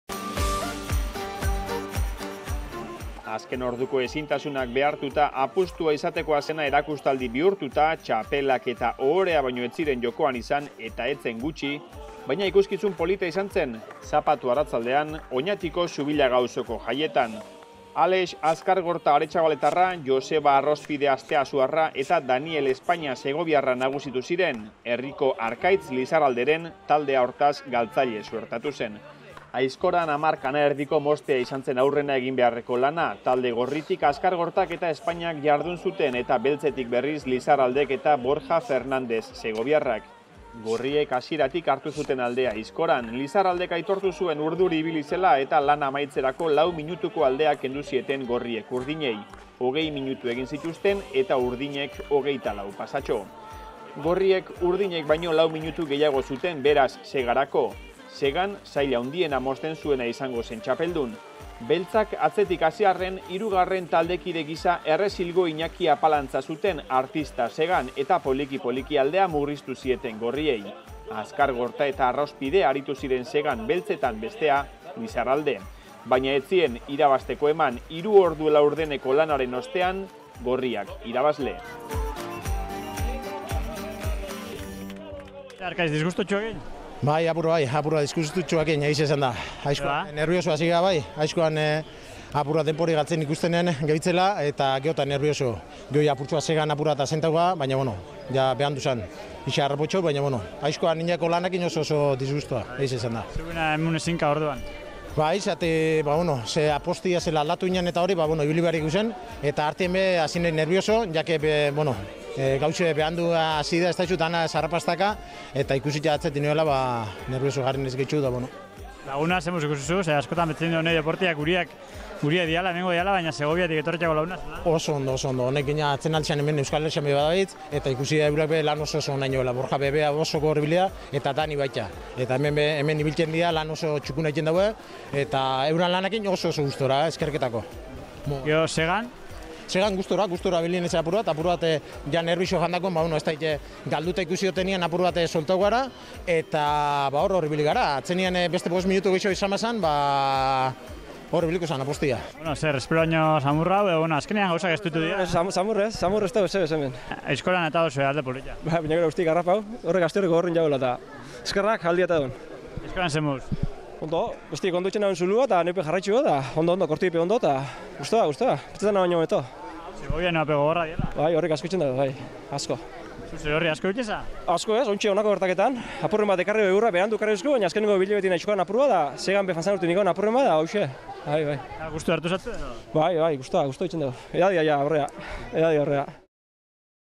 GOI 20037295
Apuesta Sega&Aizkora: Zubillaga 2018 Arkaia Speed Axe   Arkaitz Lizarralde & Alex Azkargorta